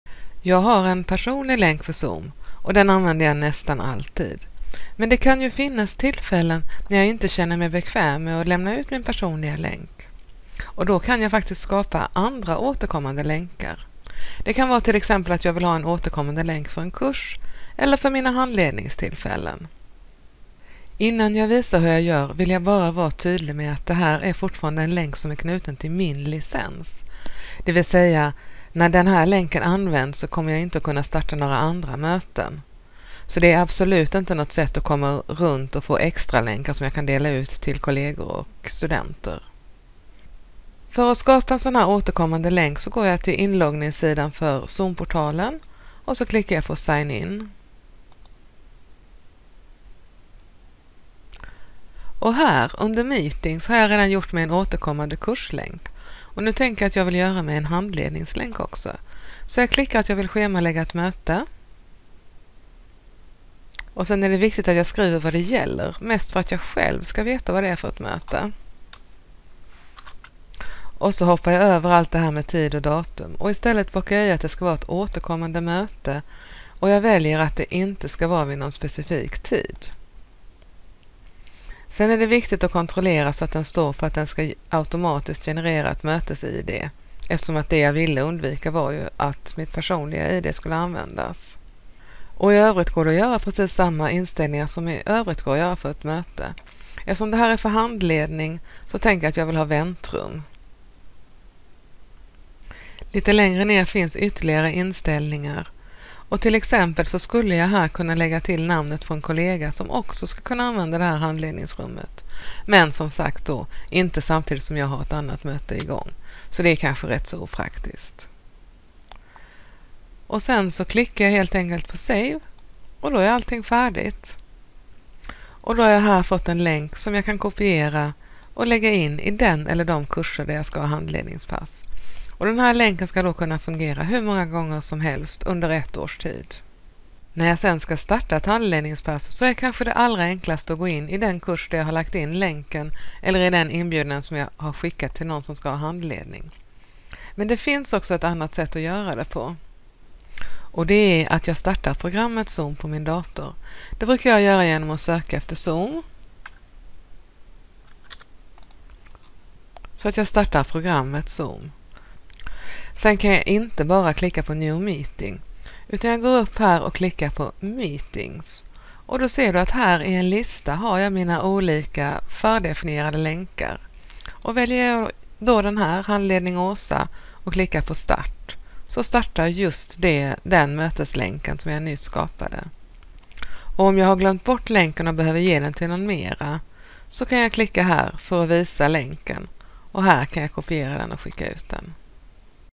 Zoom - återkommande länk som inte är min personliga
Hur du gör en återkommande länk i Zoom om du inte vill lämna ut din personliga länk. Det kan exempelvis vara användbart för handledning.

Observera att länken blir knuten till din licens. Det innebär att när den används kan du inte starta andra möten. Detta ska alltså inte ses som ett sätt att göra länkar att ge bort till kollegor eller studenter.